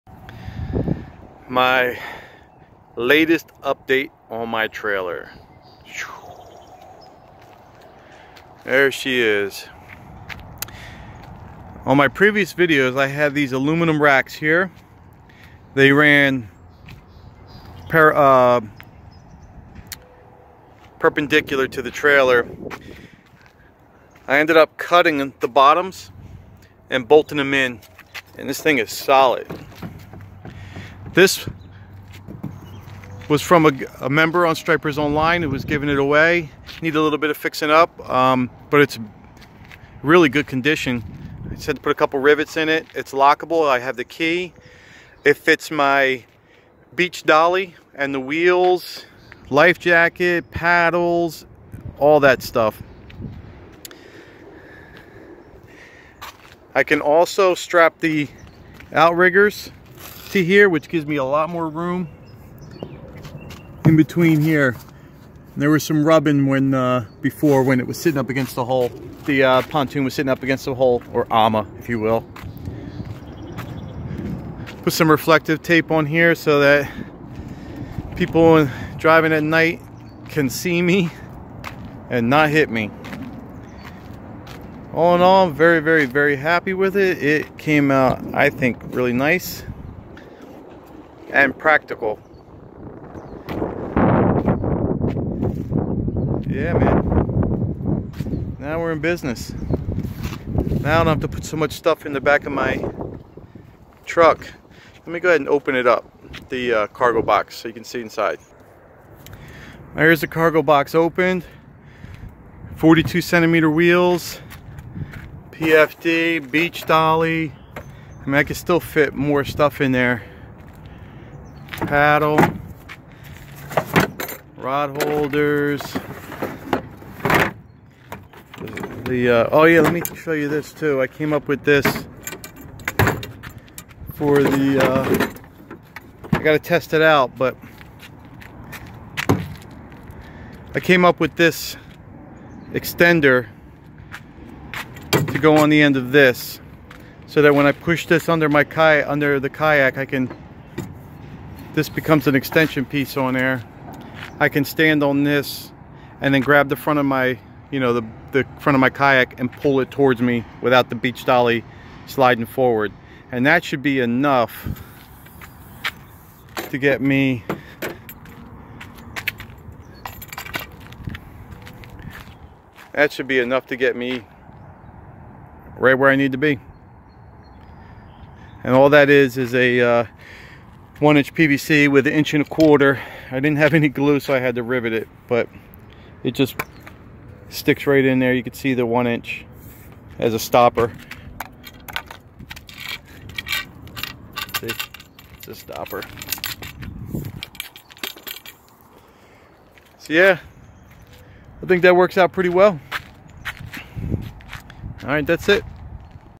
New and improved hobie adventure island trailer
Added a rack to secure the Yakima cargo box to carry all my gear.